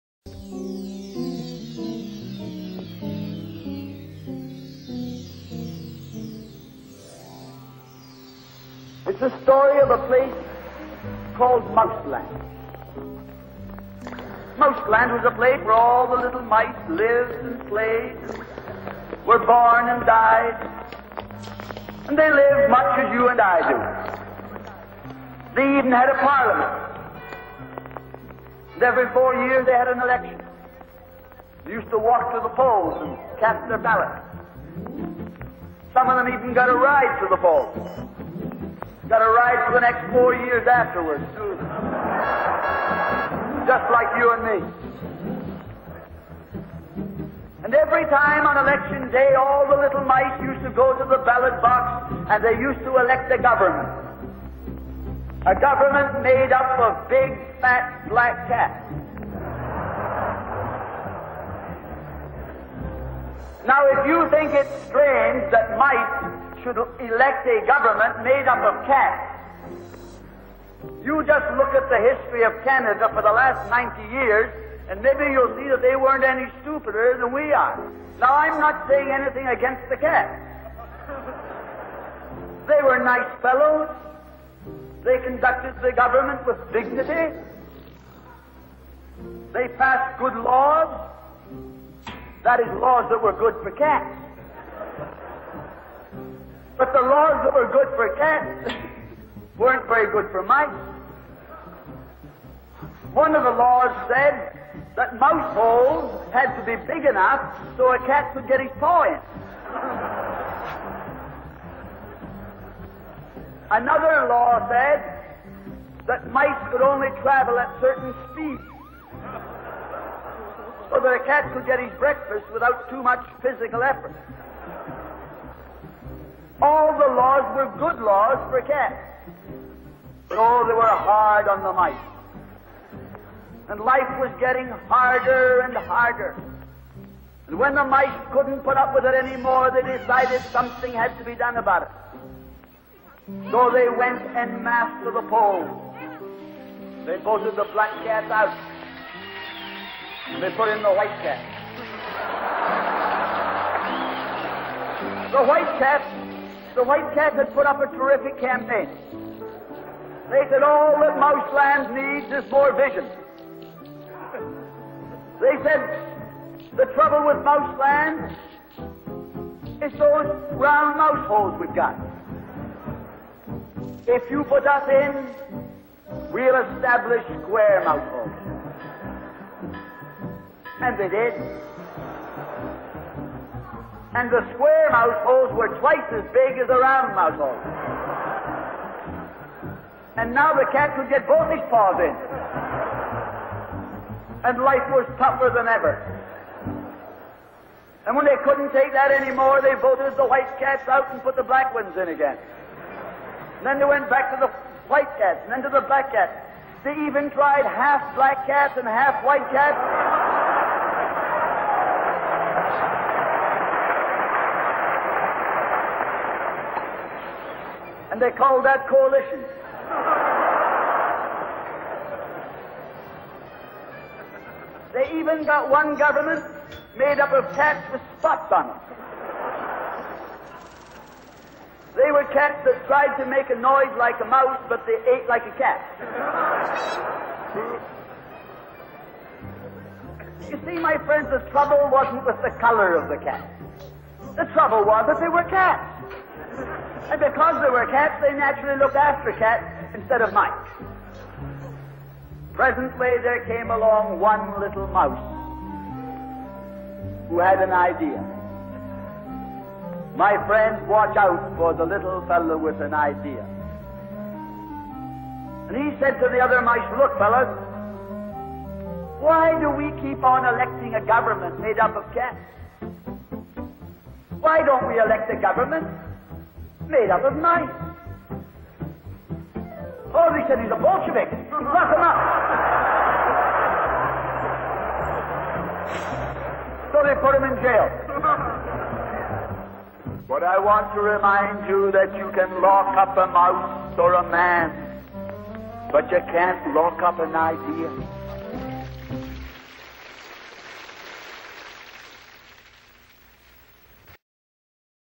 Ratolândia - Os ratos que apenas elegiam...
Uma triste mas REAL analogia à realidade vivida no nosso país!

A fábula Mouseland (em português: "Ratolândia") foi inicialmente contada por Clarence Gillis e mais tarde popularizada em discurso por Tommy Douglas, político canadiano. A fábula expressava a visão de que o sistema político canadiano estava viciado, pois oferecia aos eleitores um falso dilema: a escolha de dois partidos, dos quais nenhum representava os interesses do povo.
Na fábula, os ratos (o povo canadiano) votavam nos gatos negros (Partido Progressivo Conservador) e depois de algum tempo descobriam o quão difícil suas vidas eram. Depois votavam nos gatos brancos (Partido Liberal) e assim ficavam alternando entre os dois partidos.
Um dos ratos tem então a ideia de que os ratos deveriam formar seu próprio governo...